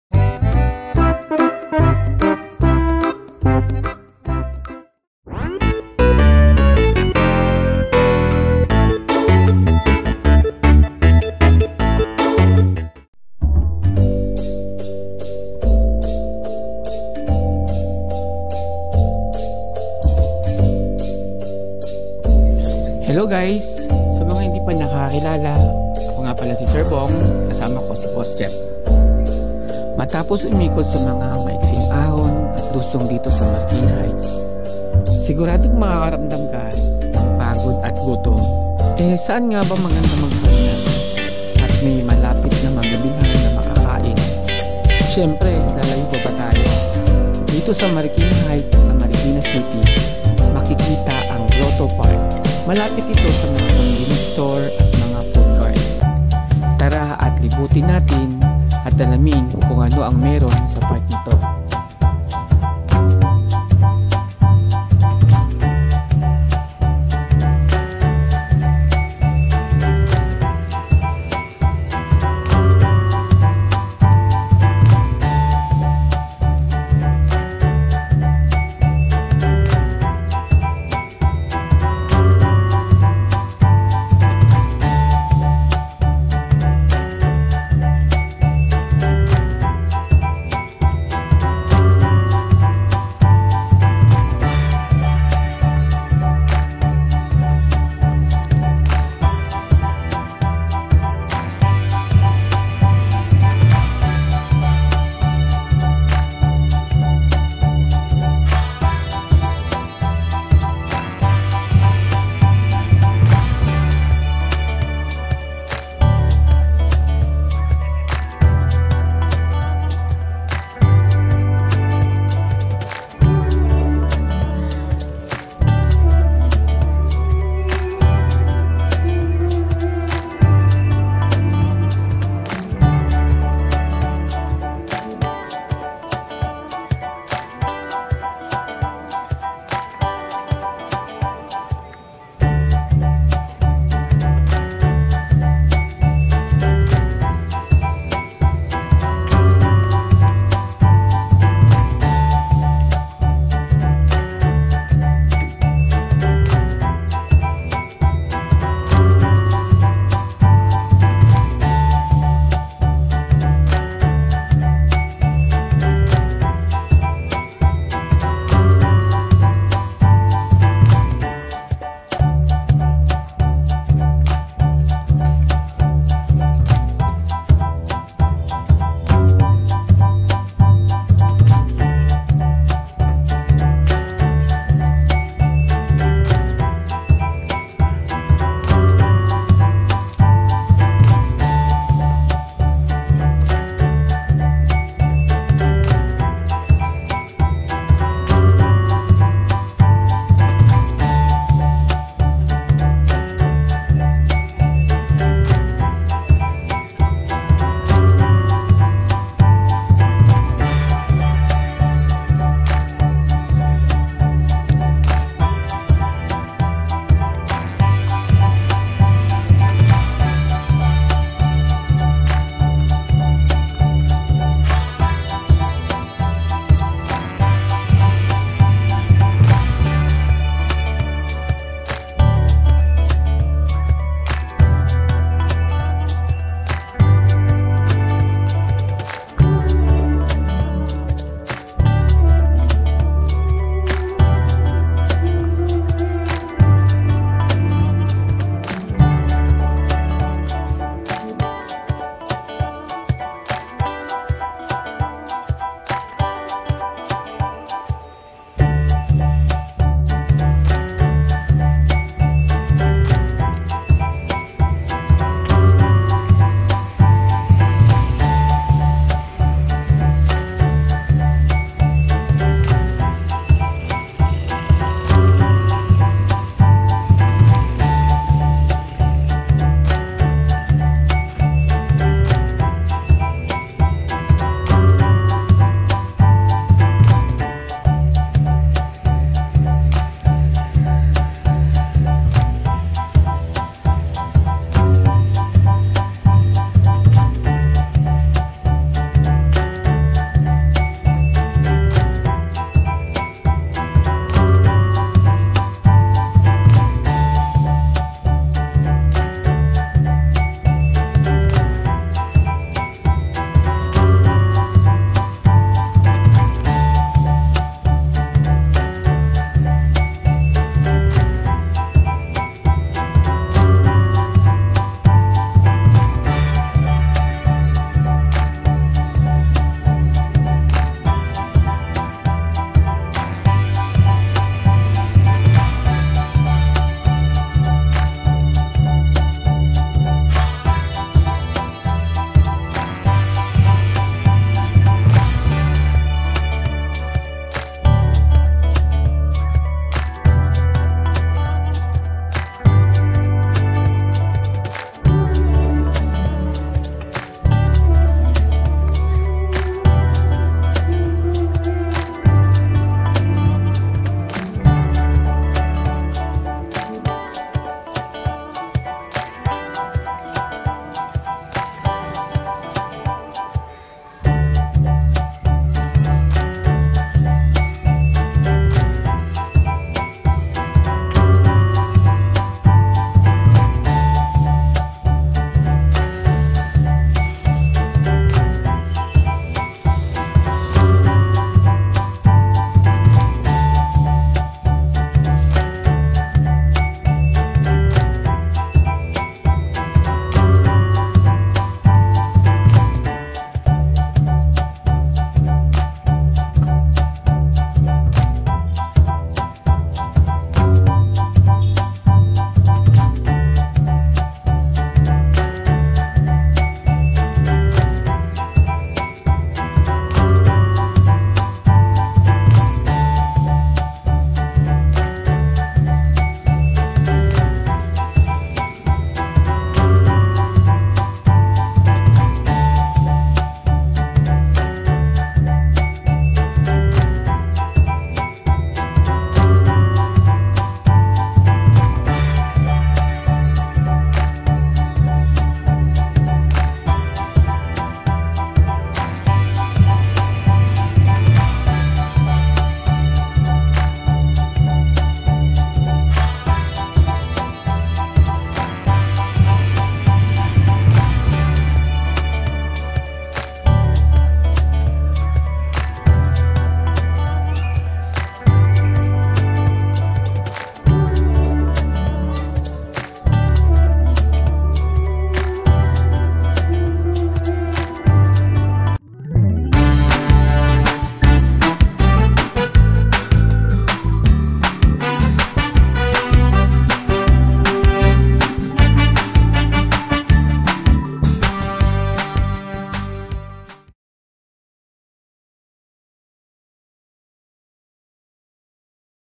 Vlog-020: Grotto Park - Marikina Heights, Marikina City
I Found a perfect Tambayan after a long bike and it is located at Marikina Heights, Marikina City